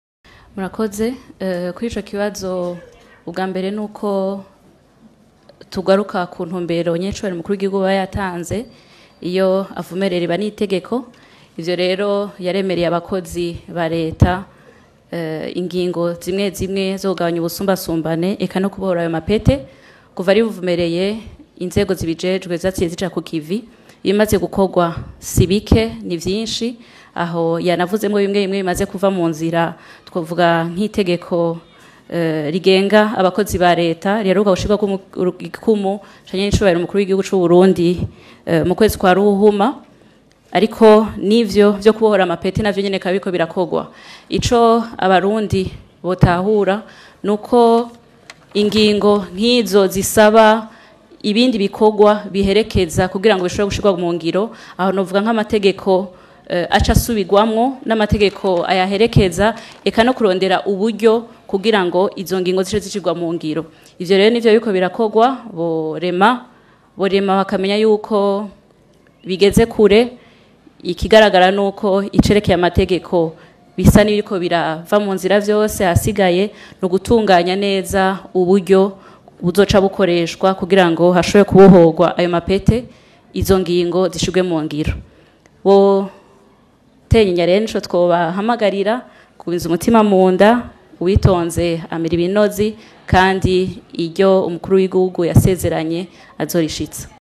Mbega amapete n'ama anale vy'abakozi bizorekurwa kuri bose ryari? Inyishu: Mutenyeye gato bizokorwa
Umuvugizi w'umukuru w'igihugu asigura ko Nk'uko sebarundi yarivumereye, bitazosubira inyuma, intambuko nyamukuru zarakozwe, hasigaye ko zija mu ngiro. Arabahumuriza nti murindire mwizeye ko biriko birakorwa.